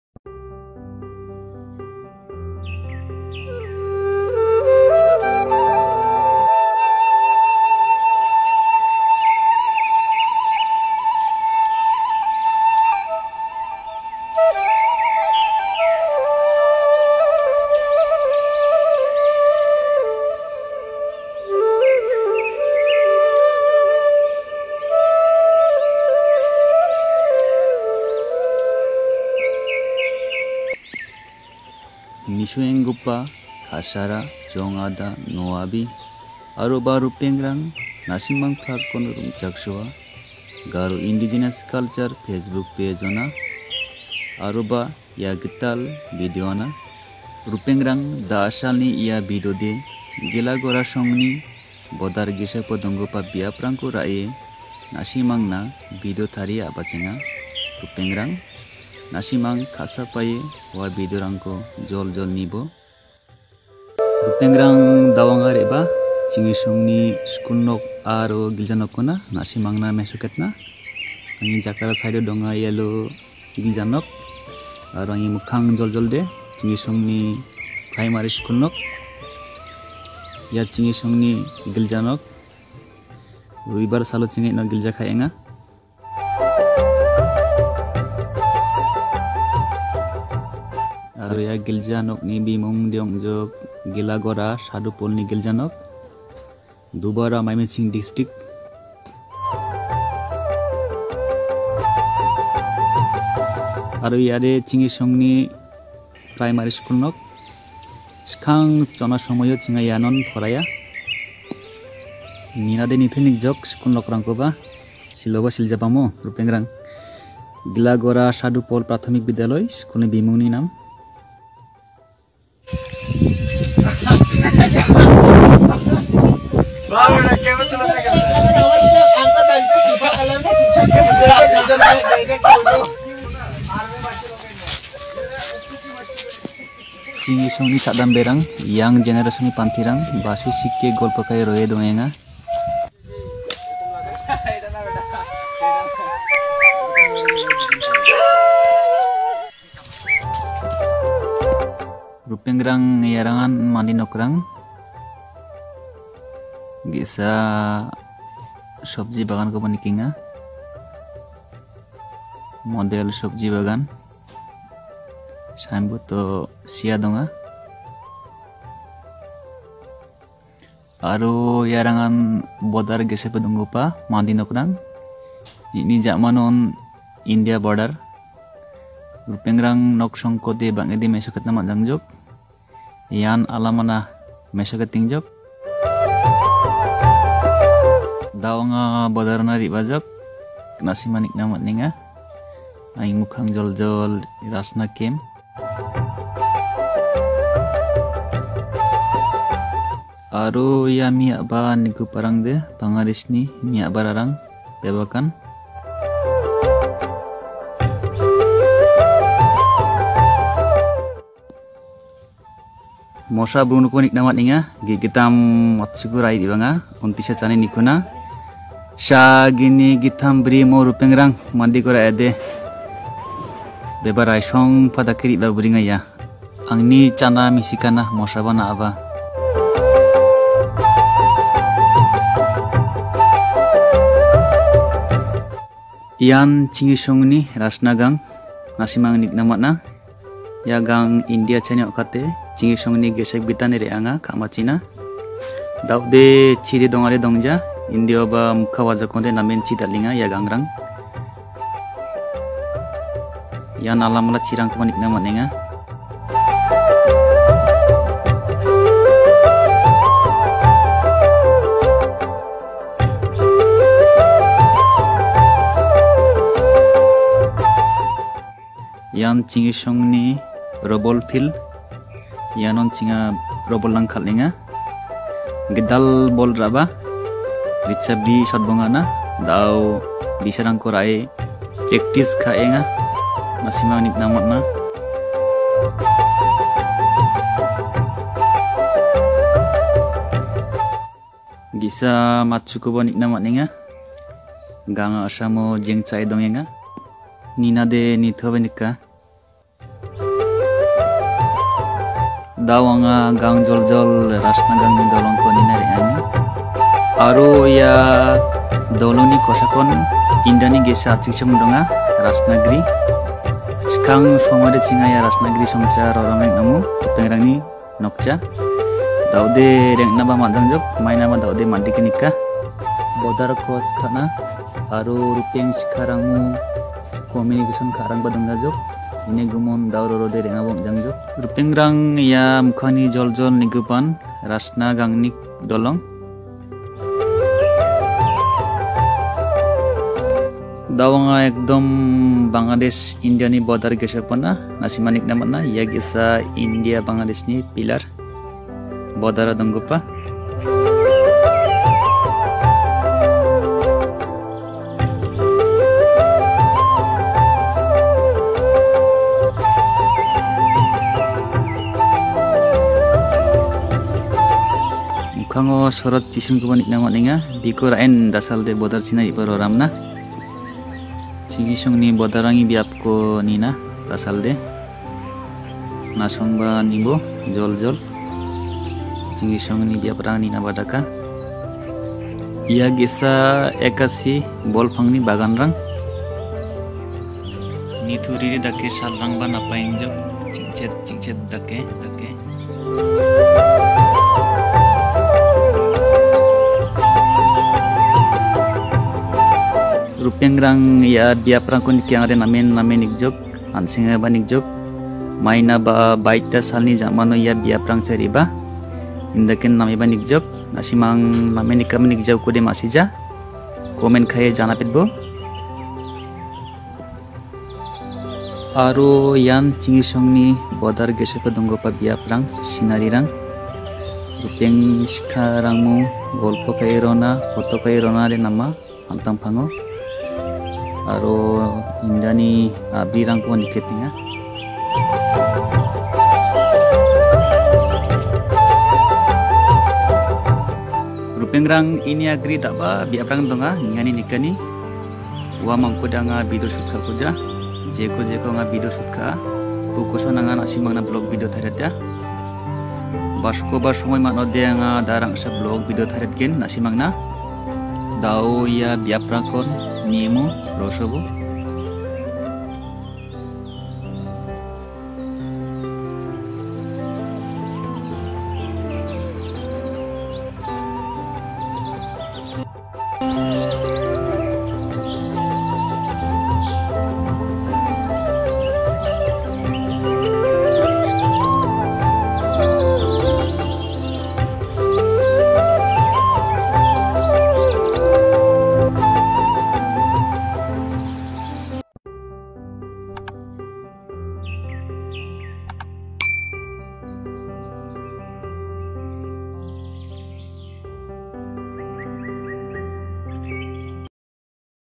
Natural Beauty ৷ Village Scenery View ৷ Beautiful Scenery Of Ghilagora
Ripengrang,Ia Video.de Ghilagora Asong Aro Bodar Gesepo Dong gipa Bi.ap Rangko Ra.e. Kandike Kagipa Gesa Vlog Video. Nasimangni chanchigipa aro nambegipa comments aro like kae janapetbo . Mikangchi jeno aro nambegipa videoko tarina ma.na .